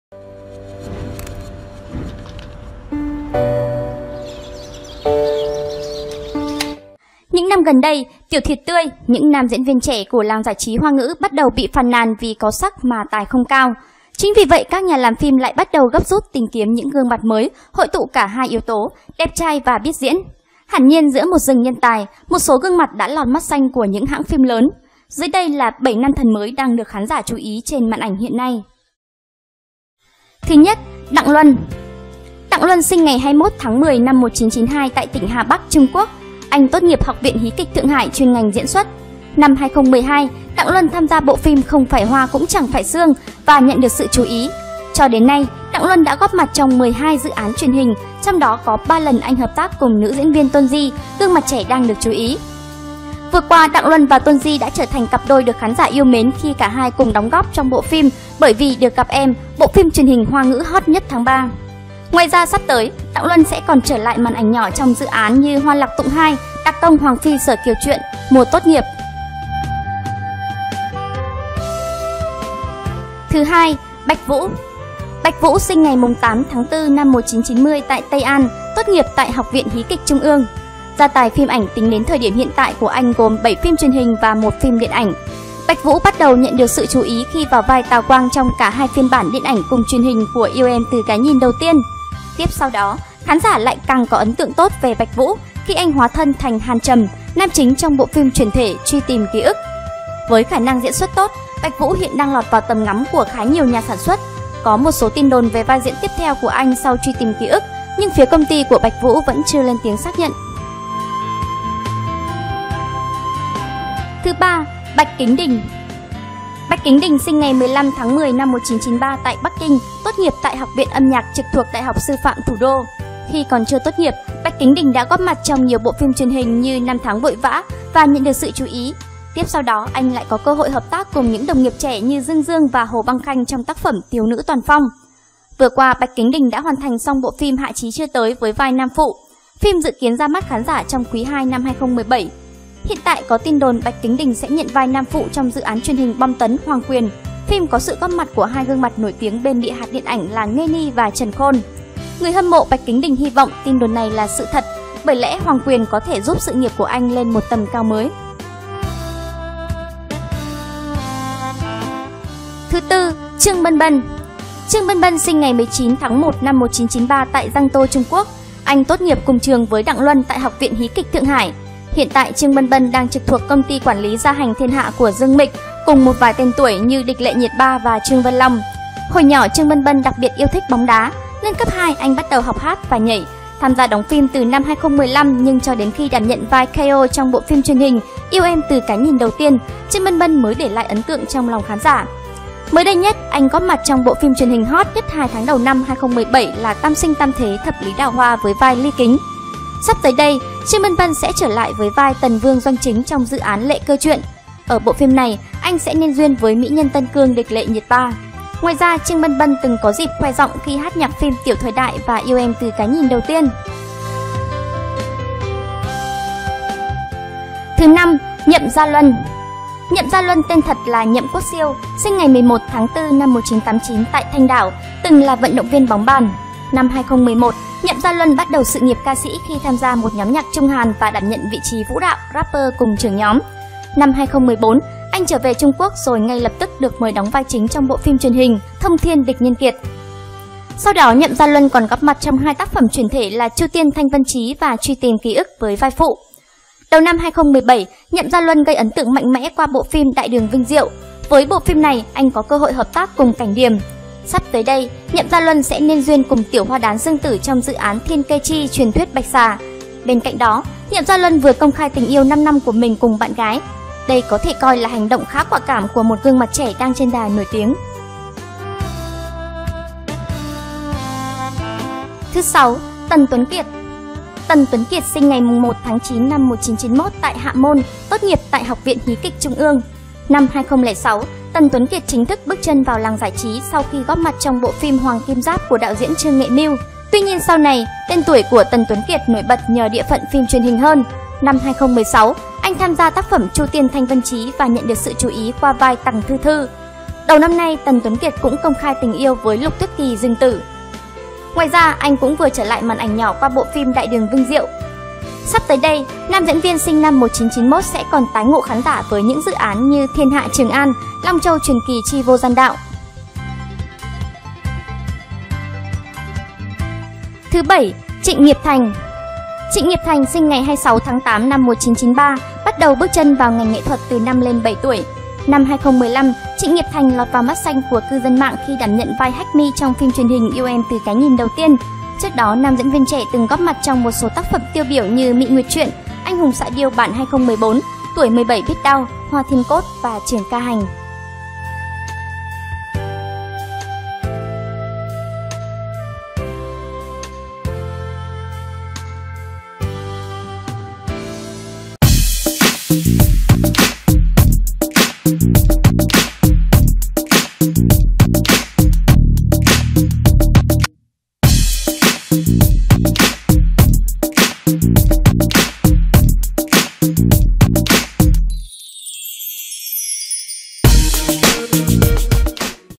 Điểm danh 7 nam thần mới vừa đẹp trai lại vừa biết diễn của màn ảnh Hoa ngữ
Top Sao Hoa tổng hợp Điểm danh 7 nam thần mới vừa đẹp trai lại vừa biết diễn của màn ảnh Hoa ngữ.
[Danh sách]:
1. Đặng Luân
2. Bạch Vũ
3. Bạch Kính Đình
4. Trương Bân Bân
5. Nhậm Gia Luân
6. Tần Tuấn Kiệt
7. Trịnh Nghiệp Thành